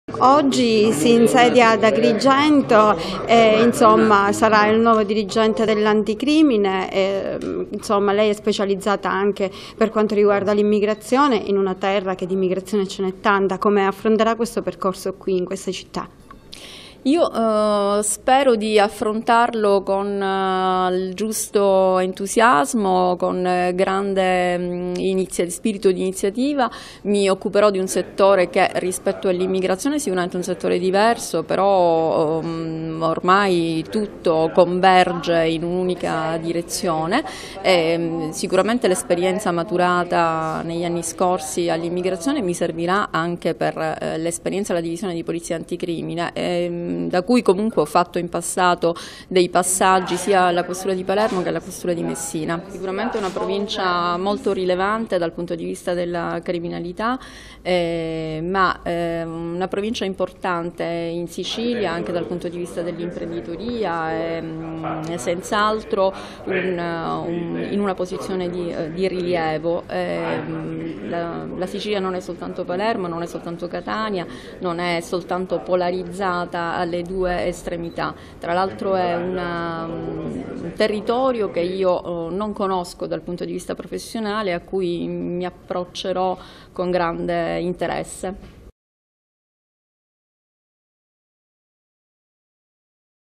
Spatola nuovo primo dirigente Polizia di Stato
Si e insediata alla questura di Agrigento Angela Maria Spatola primo dirigente di Polizia di Stato che prende il posto di Giovanni Giudice. Spatola proviene dalla Questura di Palermo occupandosi principalmente di immigrazione.